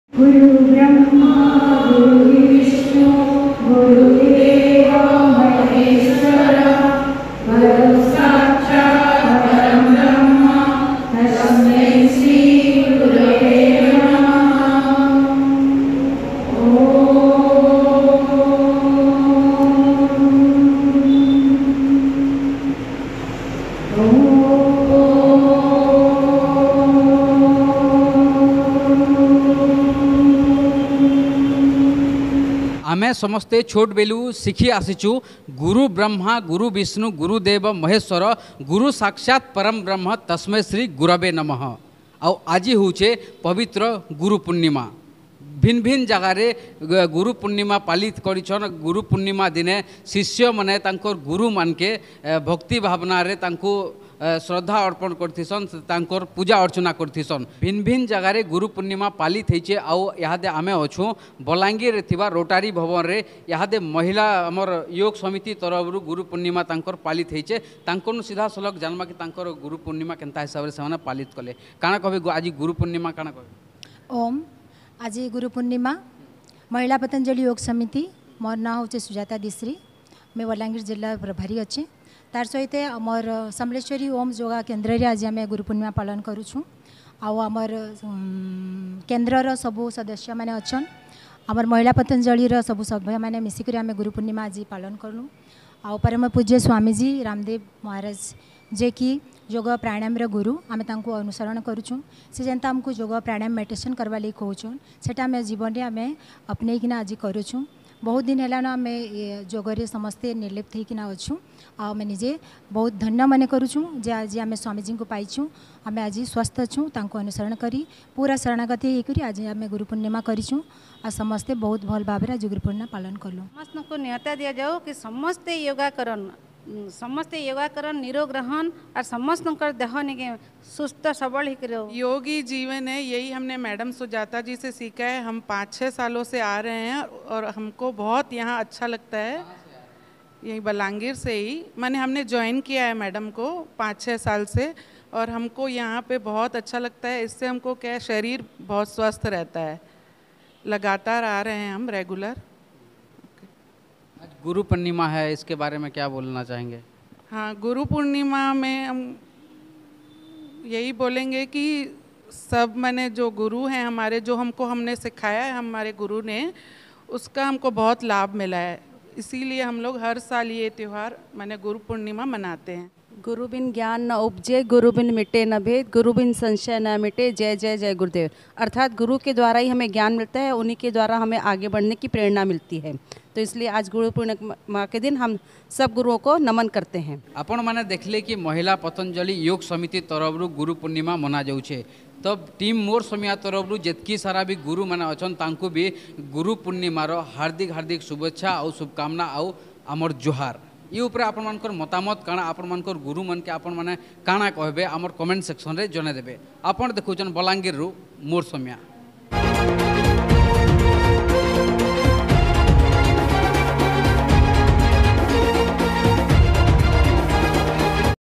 balangir news ଆଜି ଗୁରୁ ପୂର୍ଣ୍ଣିମା ପତାଞ୍ଜଳି ମହିଳା ୟୋଗ୍ ସମିତି ତରଫ ରୁ ଗୁରୁ ପୂର୍ଣ୍ଣିମା ପାଳିତ ହେଲା ।
ଅଧିକା ଯାନବାର୍ ଲାଗି ଆଉ ଆମର ସାଙ୍ଗେ ଯୁଡ଼ି ହେବାର ଲାଗି  ଅମକେ CONTACT  କରି ପାରବେ